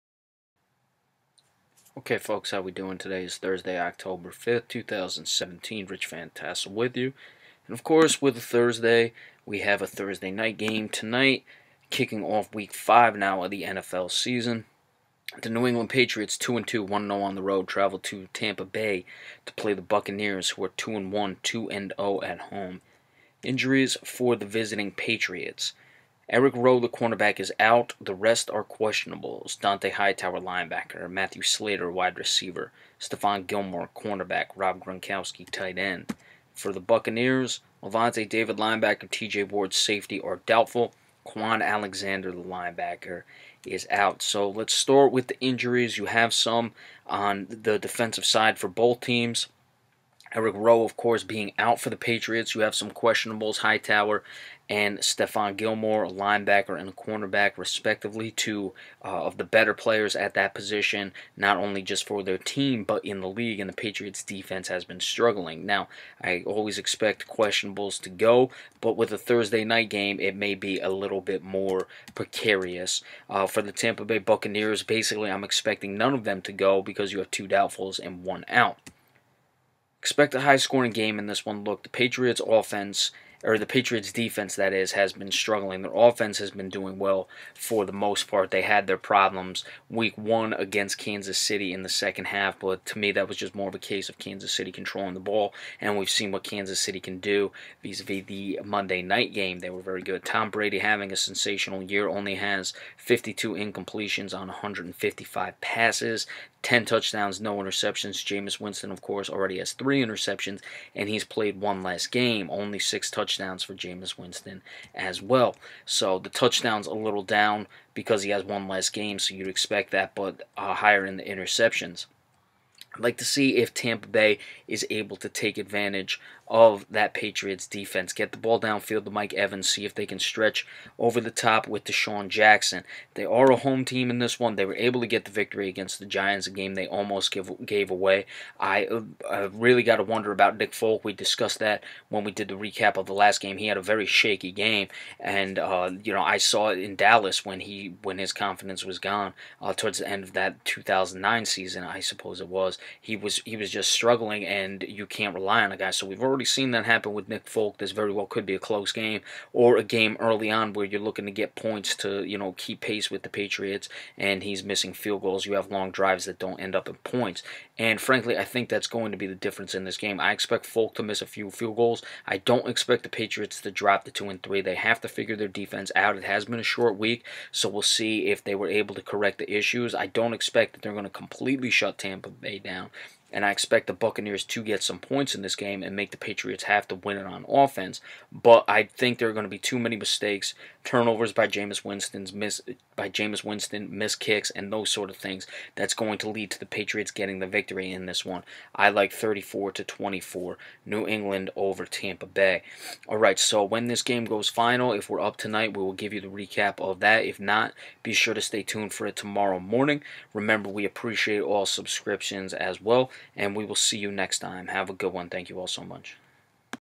Thursday Night Preview: New England Patriots vs. Tampa Bay Buccaneers
New England goes on the road with plenty of question marks as they look to avoid the 2-3 record.